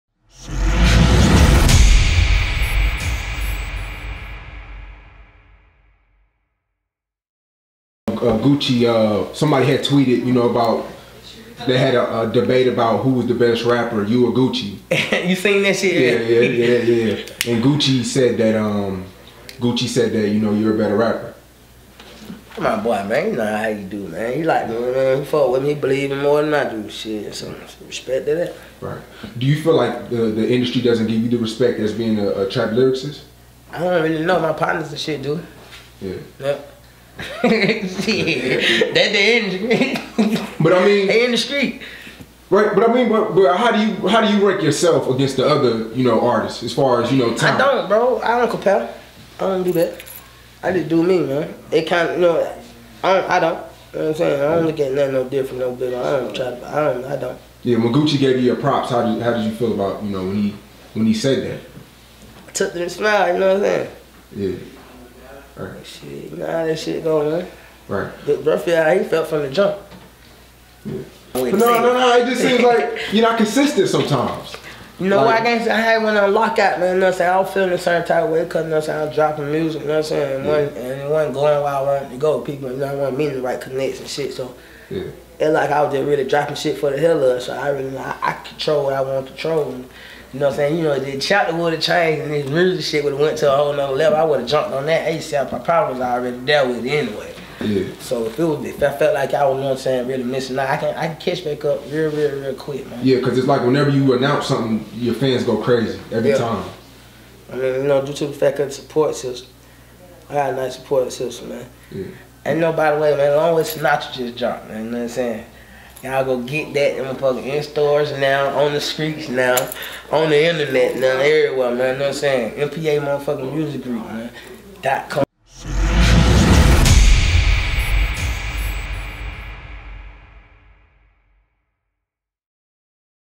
Peewee Longway: "I smiled when Gucci Mane said I'm a better rapper than him"
Atlanta artist Peewee Longway shares his thoughts on Gucci Mane saying Peewee Longway is a better artist than him.